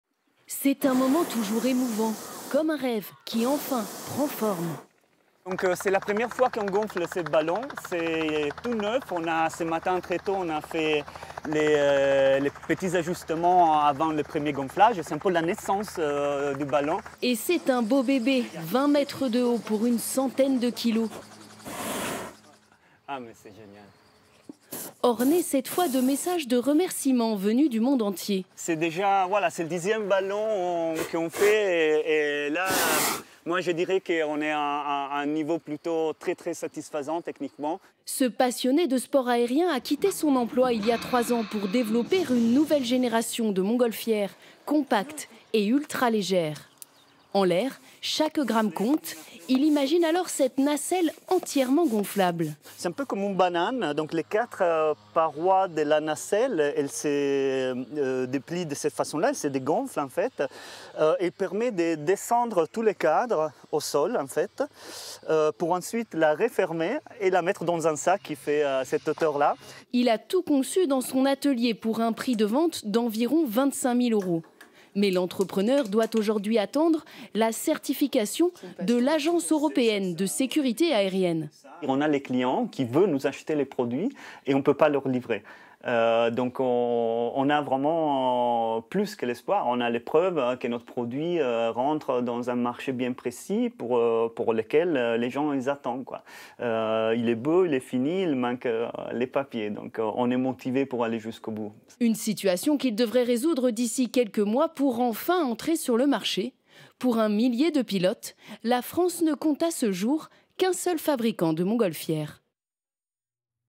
Hautes-Alpes : une montgolfière nouvelle génération à Tallard
La zone de l'aérodrome de Gap-Tallard accueille de multiples PME très innovantes dans l'aéronautique de loisir. Un entrepreneur développe notamment un nouvel modèle de montgolfière très légère et ultra-maniable.

Un reportage de : Lucie Robert et Fabien Madigou